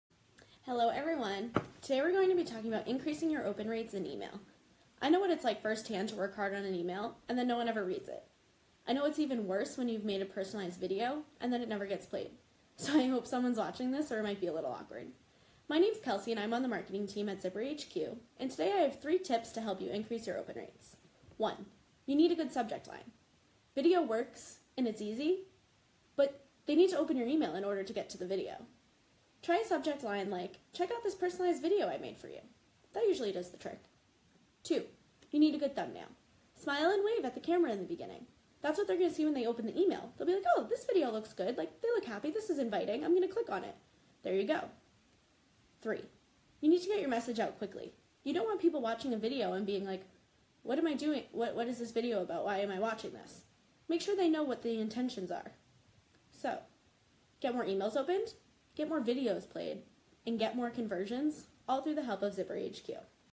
Zip Tip - Get more of your emails opened
Three quick tips to get more email opened which means more conversions for you